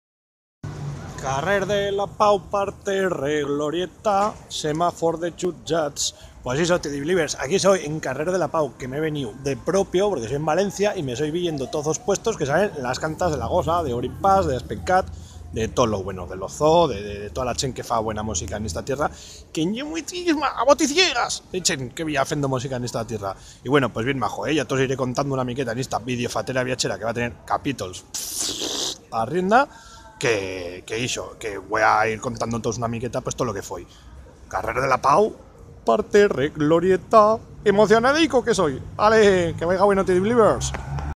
El Carrer de la Pau
Como soi mas friki que Darth Vader chugando a Magic, en o viache por Valencia m'adediqué a visitar as carreras que salen en as cancions d'as millors collas valencianas de mosica.  Videofatera 34